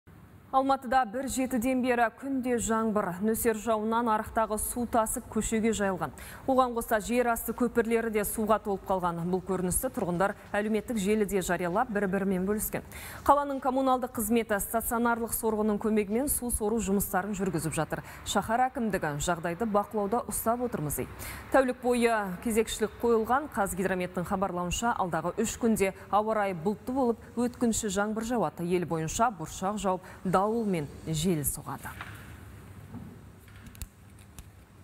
Алматыда бір аптадан бері жаңбыр жауып тұр
Алматыда бір жетіден бері күнде жаңбыр.